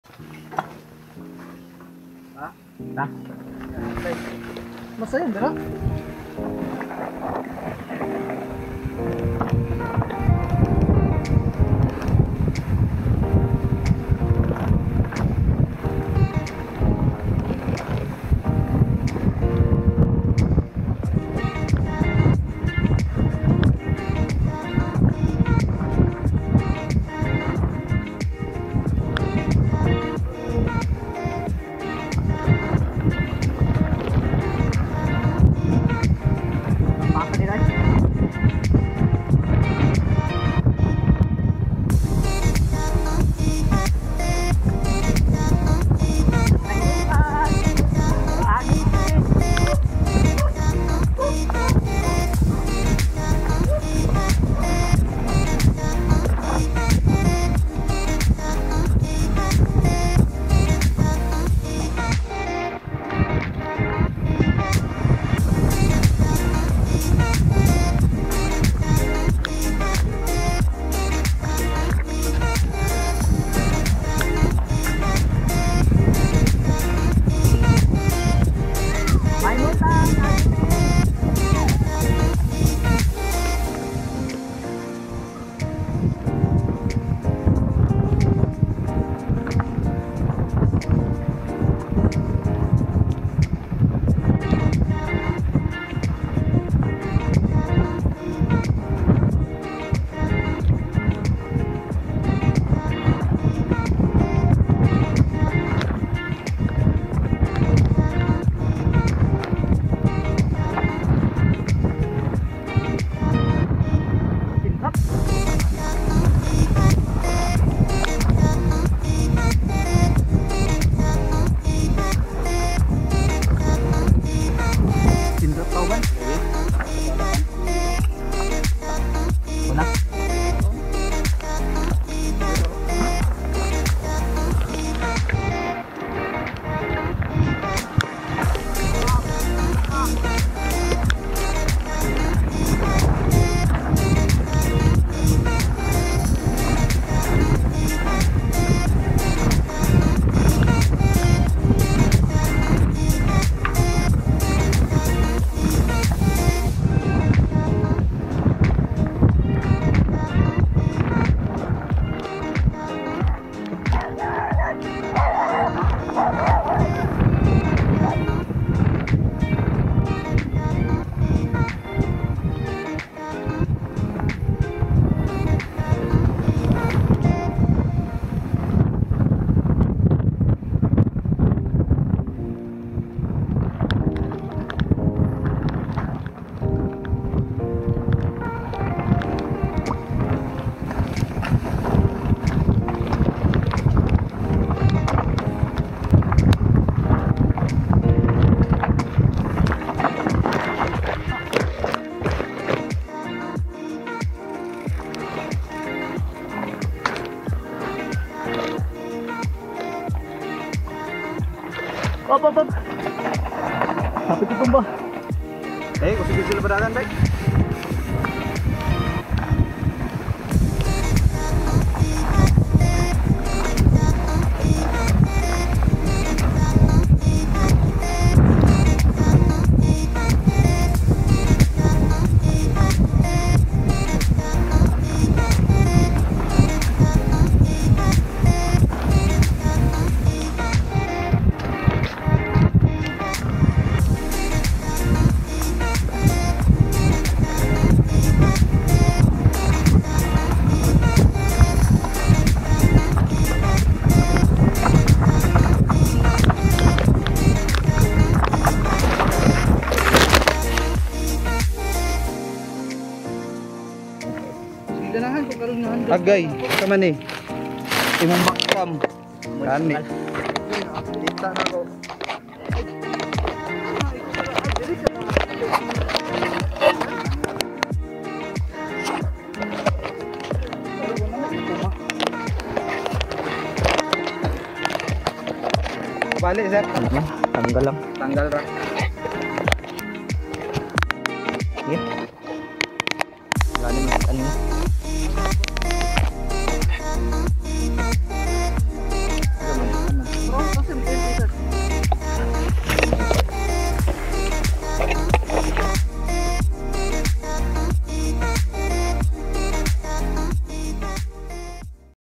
MTB single track near CDO - Dao Trail (my first crash in Dao)
Strava:
Dao DH track